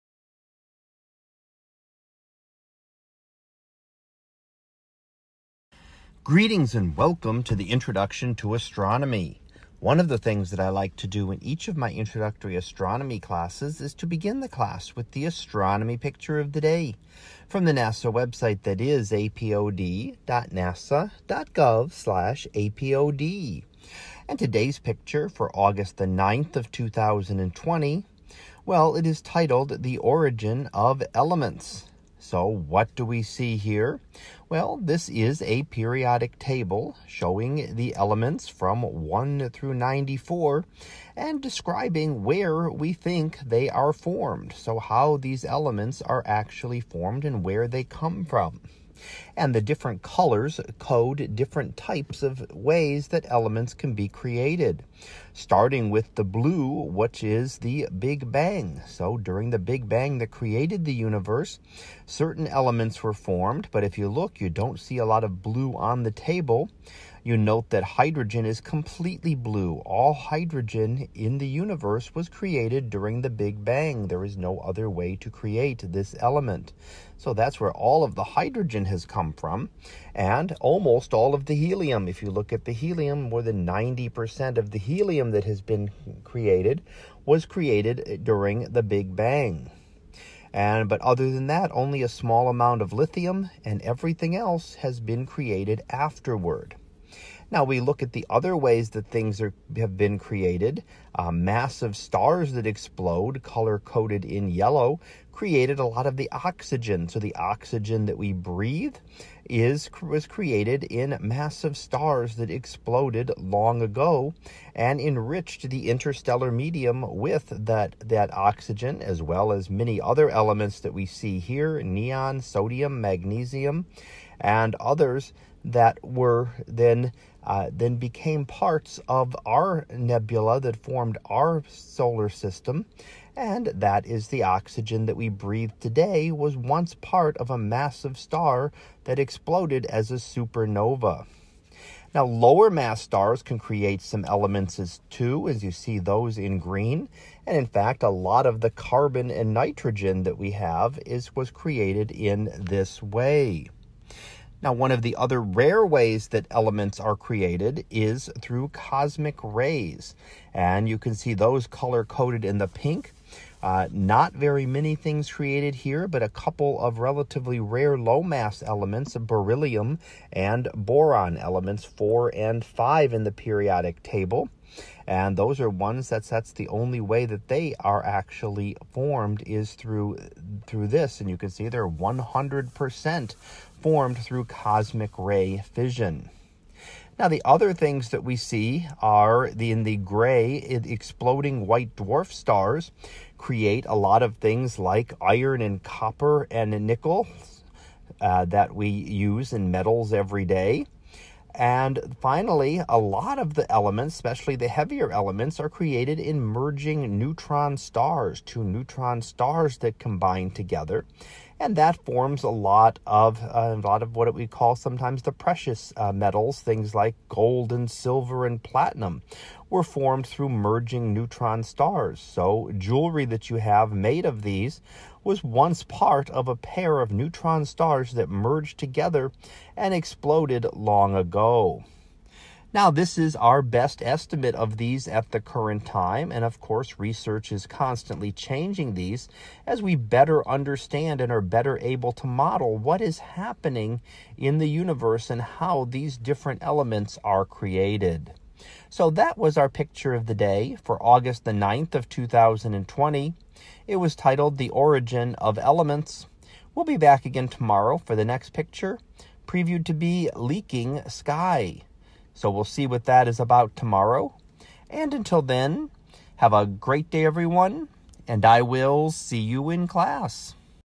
2020 August 09 - The Origin of Elements
In today's image, we see a  periodic table of the elements. This is color coded to show the origin of the elements in the universe. We see that hydrogen was formed only in the Big Bang. The oxygen we breathe was formed in the explosion of massive stars. Much of the carbon in our bodies was formed in dying low mass stars. Many precious metals - gold, silver and platinum were formed in merging neutron stars.